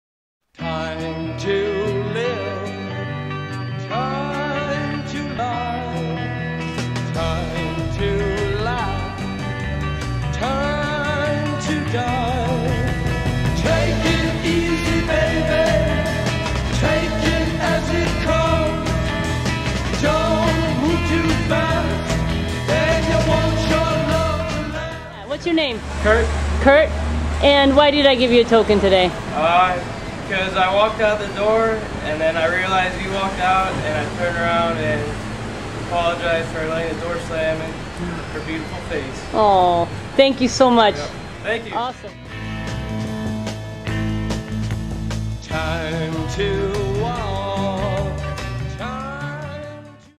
Giving Year Day 215 - Kurt
Jo Englesson, founder of TOFA, is currently living her first "Giving Year" where she will document giving a TOFA Token of Appreciation a Day for 365 days!  On Day 215 she gives a token to Kurt for opening the door for her!